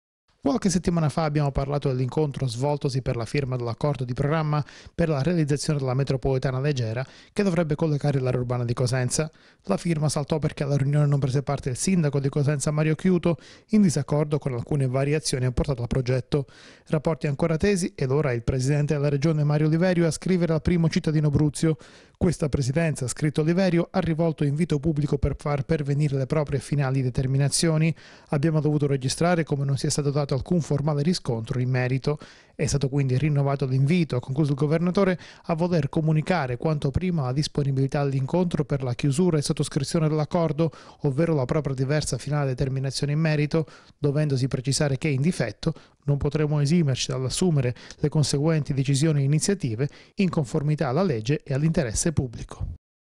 OLIVERIO SCRIVE AD OCCHIUTO
Polemiche sulla metro di Cosenza: il Presidente della Regione Calabria, Mario Oliverio, scrive a Mario Occhiuto